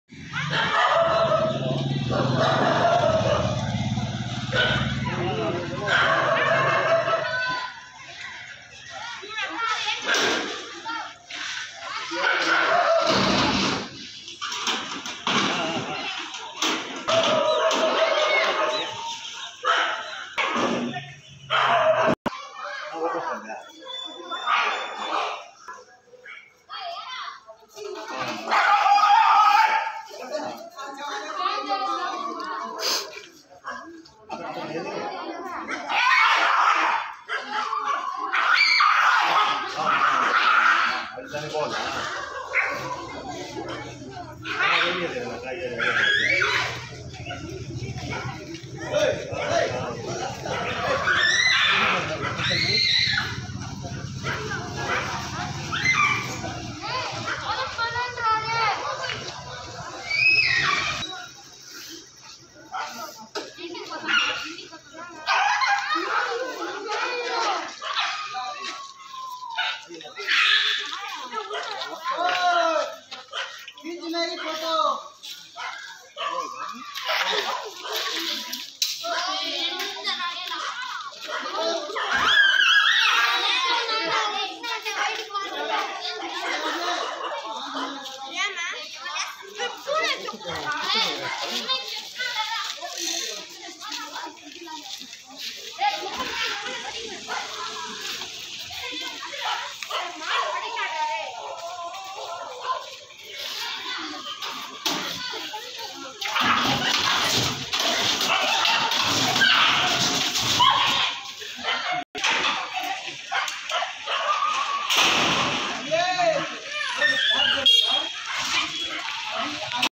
Street Dog's catching by Chittoor municipality
main intent of this video is save animals.
please save the dog's. if the dogs was catching means please let them leave from for away from the cities 🙏
please don't try to kill save the dog's life's 😥🙏